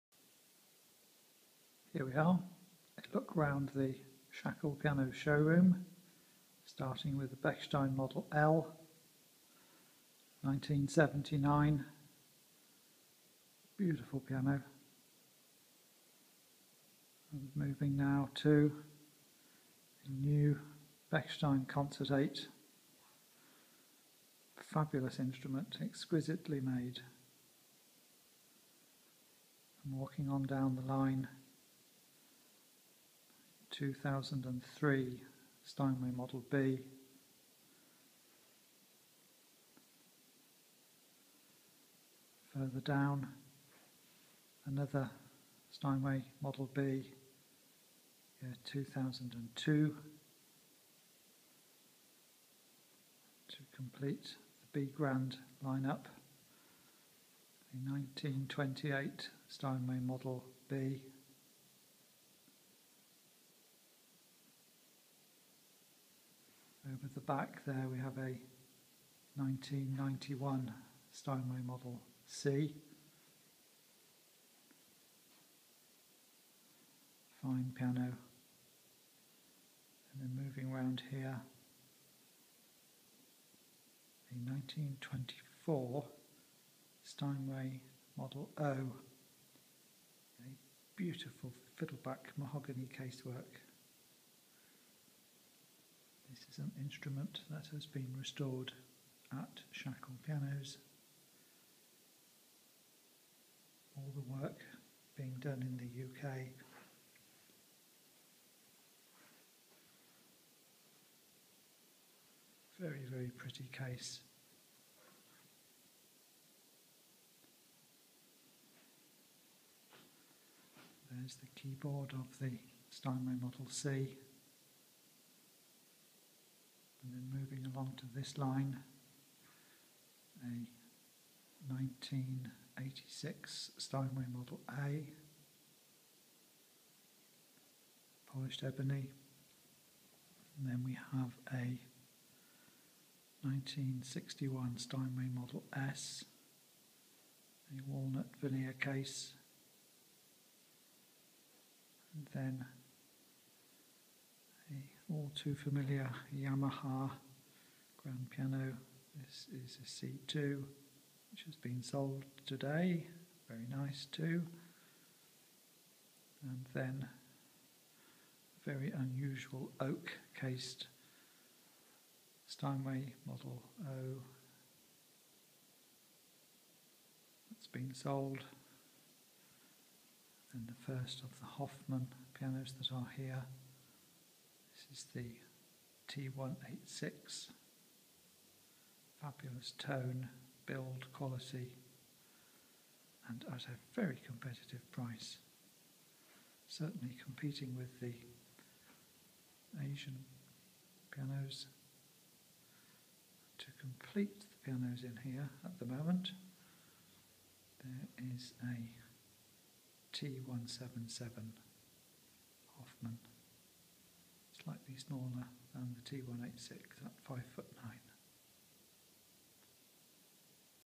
Grand piano showroom
A look round the grand piano showroom at Shackell Pianos Ltd in Witney, Oxfordshire